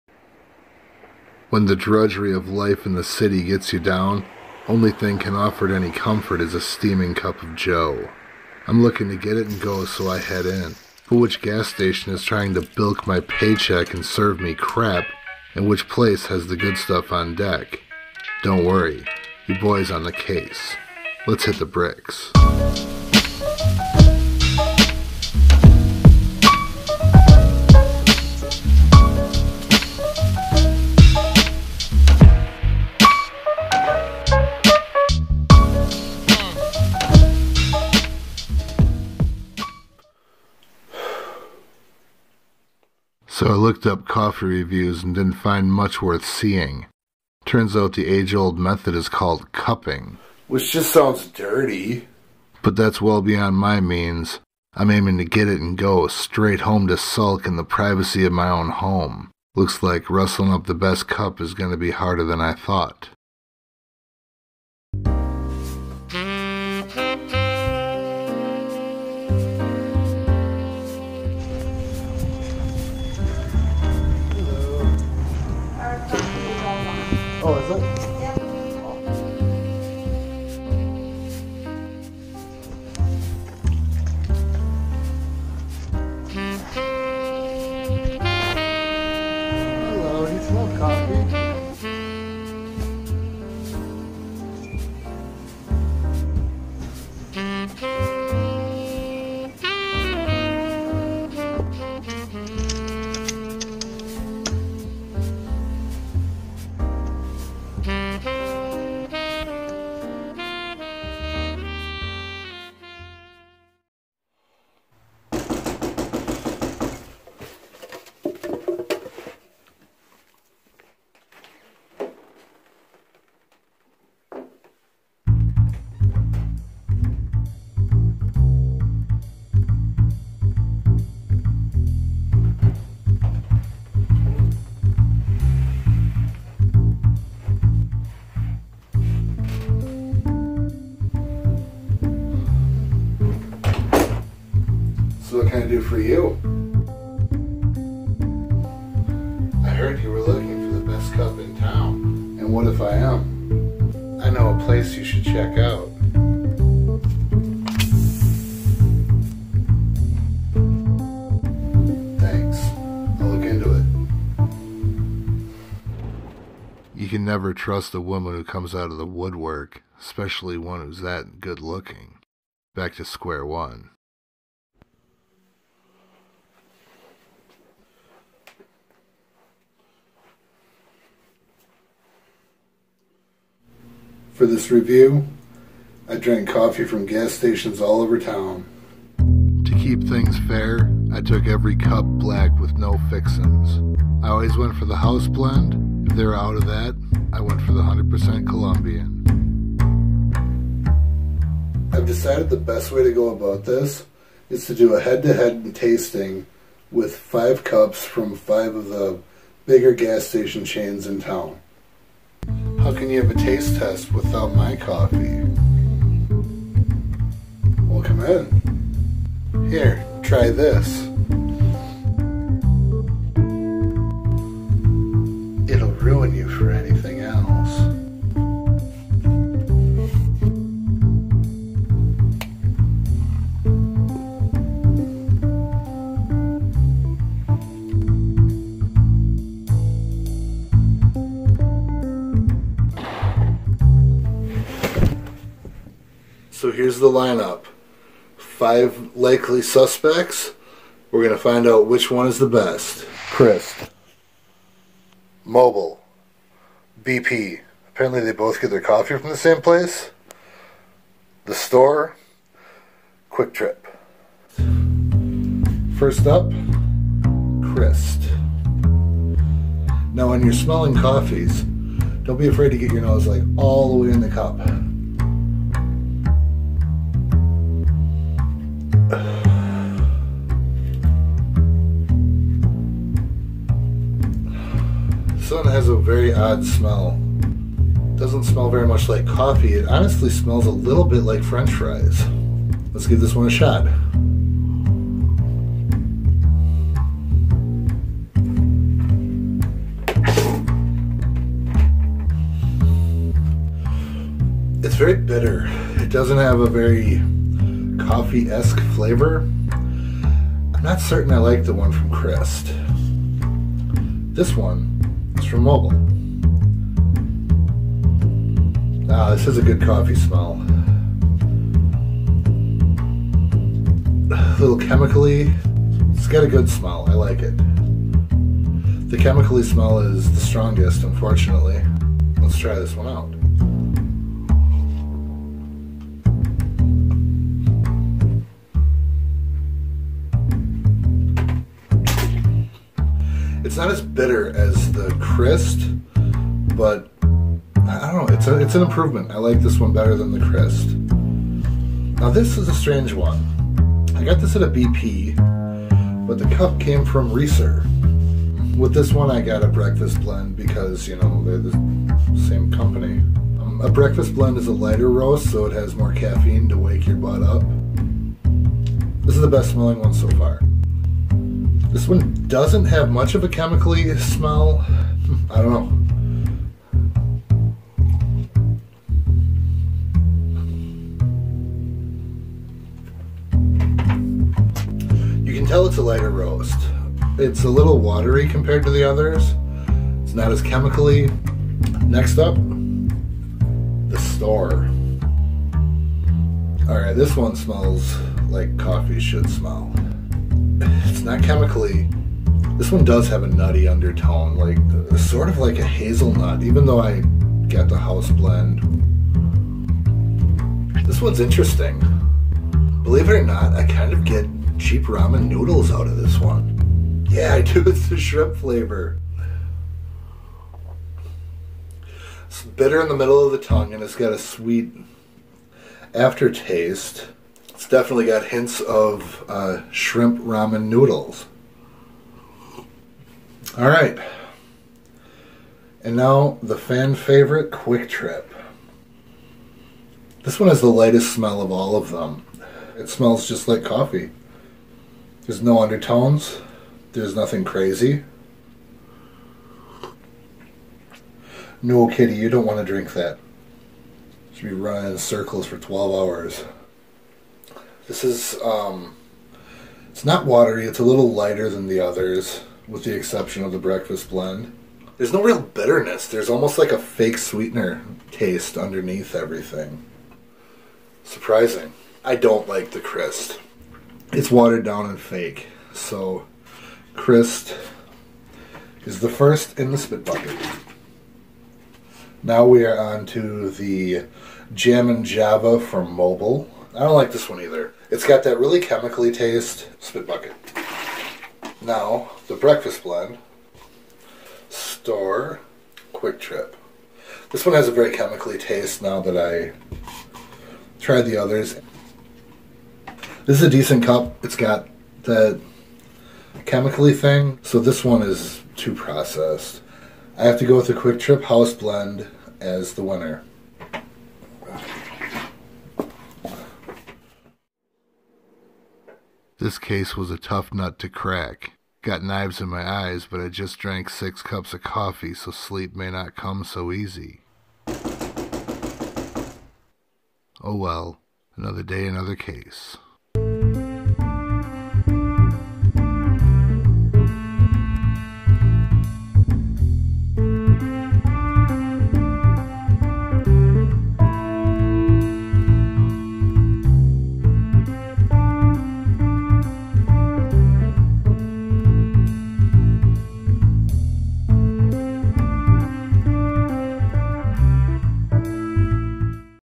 Gas Station Coffee review: 5 Cups from 5 places, head to head in a Film Noir Story
Sometimes you just need a quick cup of coffee to get your mind right. Times like these, you need it fast, and the gas station is a popular spot to pick up a cup on the go. Which place has the best one? Which place … doesn't? I mean, it's only a buck fifty, but a bunk cup can make a hairy situation all the worse. Join me on a film noir adventure to find the best gas station cup of joe in town.

Secondary Camera Person:
Randy VonBurgandy